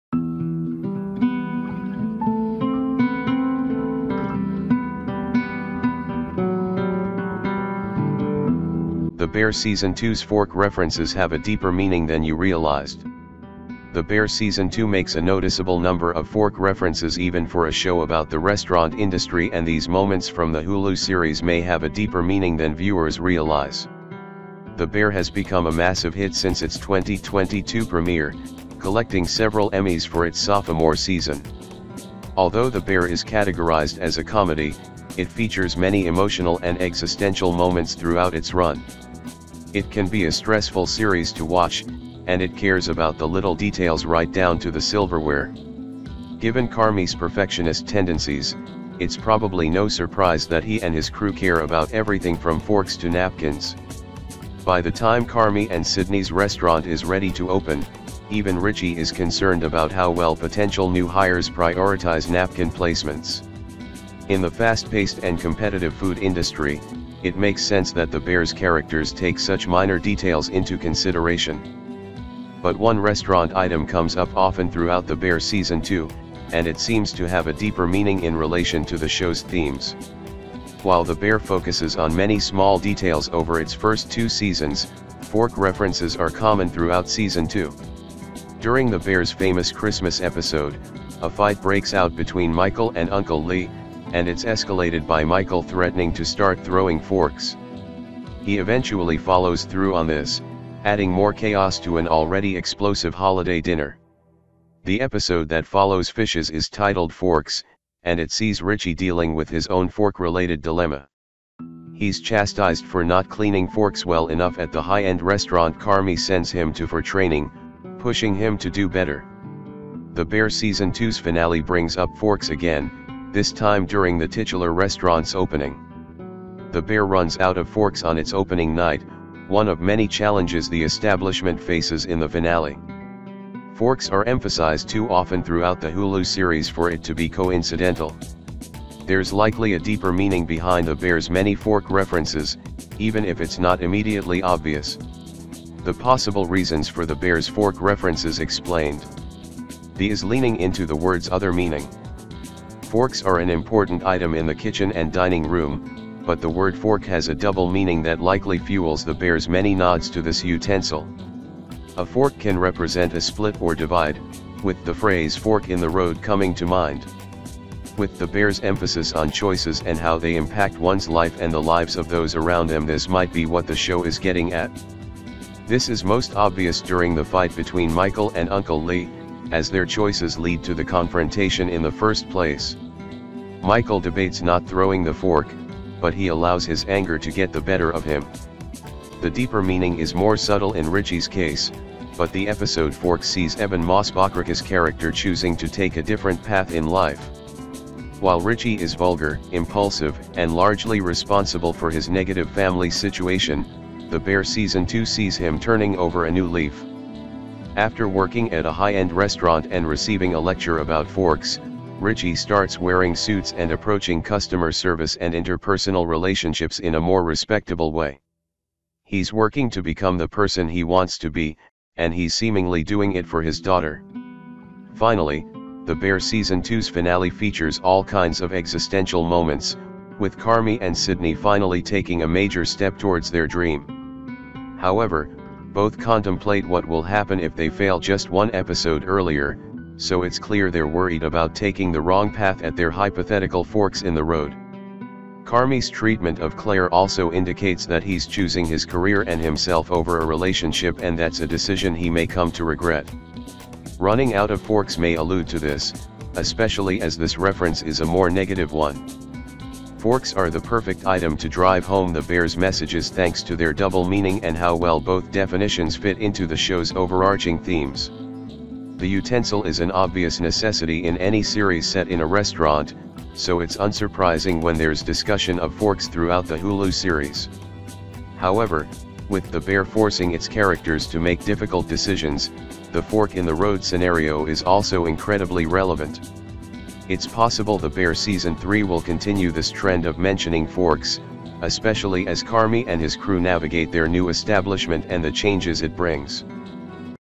🔴The Bear Season 2's Fork References Have A Deeper Meaning Than You Realized🔴
The Bear season 2 makes a noticeable number of fork references — even for a show about the restaurant industry — and these moments from the Hulu series may have a deeper meaning than viewers realize. The Bear has become a massive hit since its 2022 premiere, collecting several Emmys for its sophomore season. Although The Bear is categorized as a comedy, it features many emotional and existential moments throughout its run. It can be a stressful series to watch, and it cares about the little details — right down to the silverware.

Given Carmy's perfectionist tendencies, it's probably no surprise that he and his crew care about everything from forks to napkins. By the time Carmy and Sydney's restaurant is ready to open, even Richie is concerned about how well potential new hires prioritize napkin placements. In the fast-paced and competitive food industry, it makes sense that The Bear's characters take such minor details into consideration. But one restaurant item comes up often throughout The Bear season 2, and it seems to have a deeper meaning in relation to the show's themes.

While The Bear focuses on many small details over its first two seasons, fork references are common throughout season 2. During The Bear's famous Christmas episode, a fight breaks out between Michael and Uncle Lee, and it's escalated by Michael threatening to start throwing forks. He eventually follows through on this, adding more chaos to an already explosive holiday dinner. The episode that follows "Fishes" is titled "Forks," and it sees Richie dealing with his own fork-related dilemma. He's chastised for not cleaning forks well enough at the high-end restaurant Carmy sends him to for training, pushing him to do better.

The Bear season 2's finale brings up forks again, this time during the titular restaurant's opening. The Bear runs out of forks on its opening night, one of many challenges the establishment faces in the finale. Forks are emphasized too often throughout the Hulu series for it to be coincidental. There's likely a deeper meaning behind The Bear's many fork references, even if it's not immediately obvious.

The Possible Reasons For The Bear's Fork References Explained
The  Is Leaning Into The Word's Other Meaning

Forks are an important item in the kitchen and dining room, but the word "fork" has a double meaning that likely fuels The Bear's many nods to this utensil. A fork can represent a split or divide, with the phrase "fork in the road" coming to mind. With The Bear's emphasis on choices and how they impact one's life — and the lives of those around them — this might be what the show is getting at. This is most obvious during the fight between Michael and Uncle Lee, as their choices lead to the confrontation in the first place. Michael debates not throwing the fork, but he allows his anger to get the better of him.

The deeper meaning is more subtle in Richie's case, but the episode "Forks" sees Ebon Moss-Bachrach's character choosing to take a different path in life. While Richie is vulgar, impulsive, and largely responsible for his negative family situation, The Bear season 2 sees him turning over a new leaf. After working at a high-end restaurant and receiving a lecture about forks, Richie starts wearing suits and approaching customer service and interpersonal relationships in a more respectable way. He's working to become the person he wants to be, and he's seemingly doing it for his daughter.

Finally, The Bear season 2's finale features all kinds of existential moments, with Carmy and Sydney finally taking a major step towards their dream. However, both contemplate what will happen if they fail just one episode earlier, so it's clear they're worried about taking the wrong path at their hypothetical forks in the road. Carmy's treatment of Claire also indicates that he's choosing his career and himself over a relationship — and that's a decision he may come to regret. Running out of forks may allude to this, especially as this reference is a more negative one.

Forks are the perfect item to drive home The Bear's messages thanks to their double meaning and how well both definitions fit into the show's overarching themes. The utensil is an obvious necessity in any series set in a restaurant, so it's unsurprising when there's discussion of forks throughout the Hulu series. However, with The Bear forcing its characters to make difficult decisions, the fork in the road scenario is also incredibly relevant. It's possible The Bear season 3 will continue this trend of mentioning forks, especially as Carmy and his crew navigate their new establishment and the changes it brings.